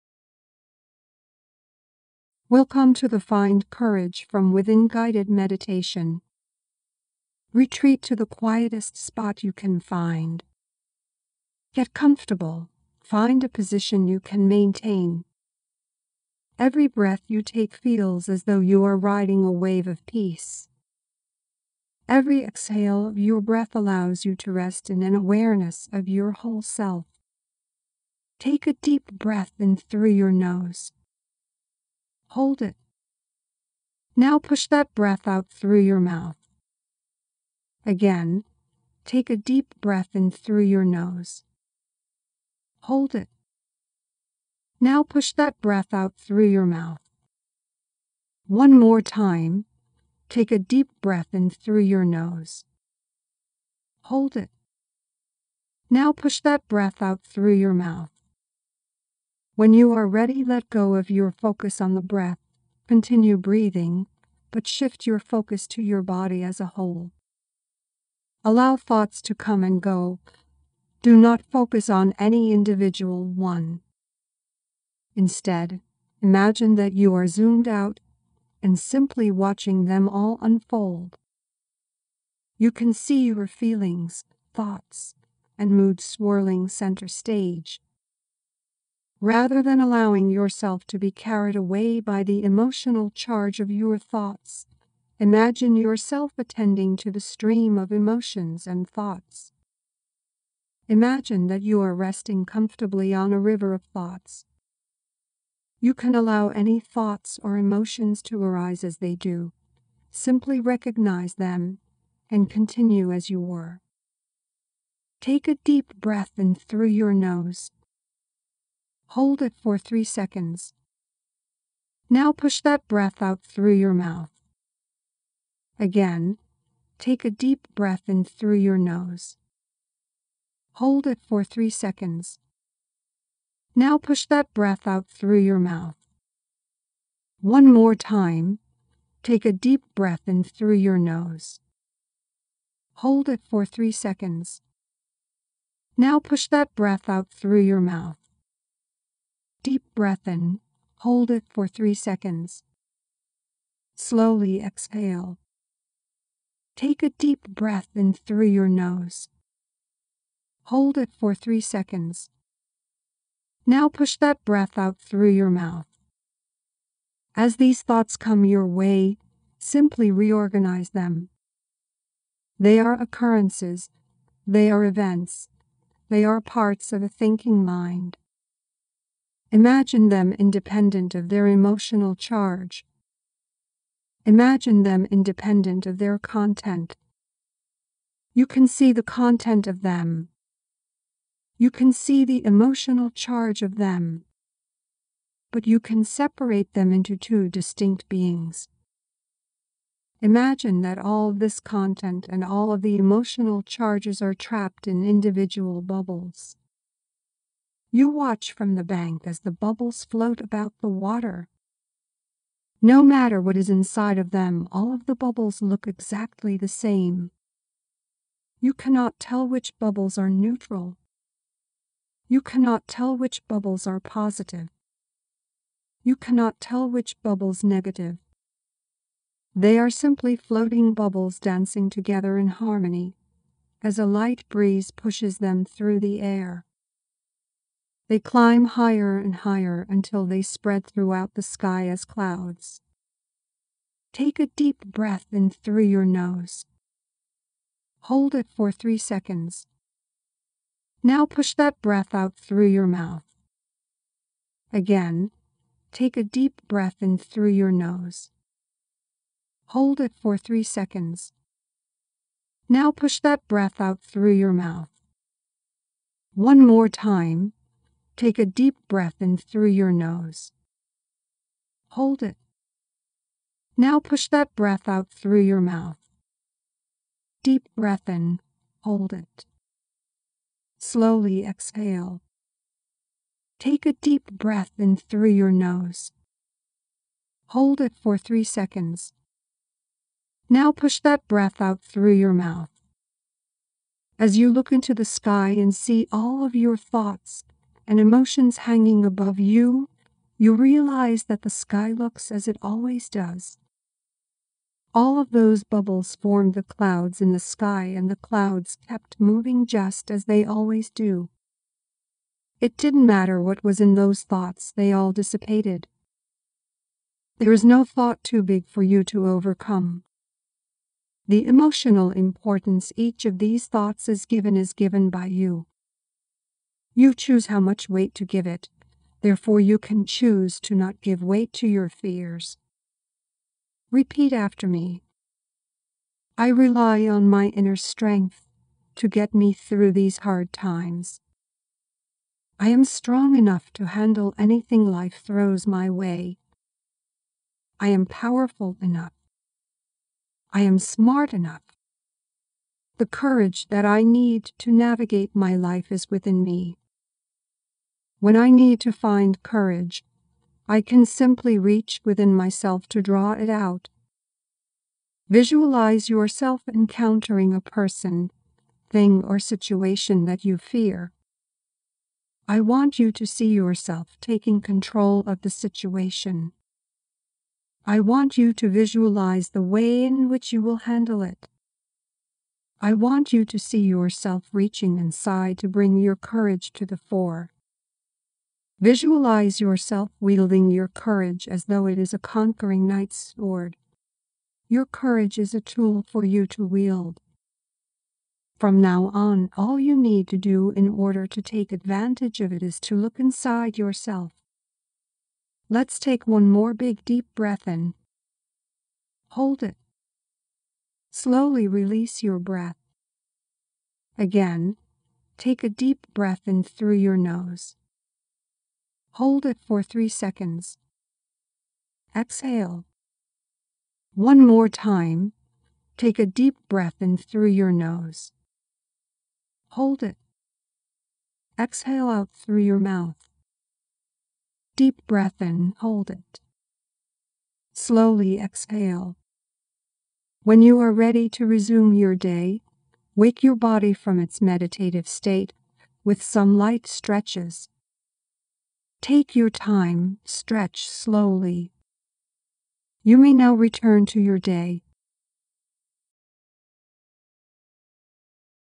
Guided Meditation: Find Courage From Within
Welcome to the Find Courage From Within guided meditation. Retreat to the quietest spot you can find. Get comfortable, find a position you can maintain.

Every breath you take feels as though you are riding a wave of peace. Every exhale of your breath allows you to rest in an awareness of your whole self. Take a deep breath in through your nose. Hold it. Now push that breath out through your mouth. Again, take a deep breath in through your nose. Hold it.
Now push that breath out through your mouth .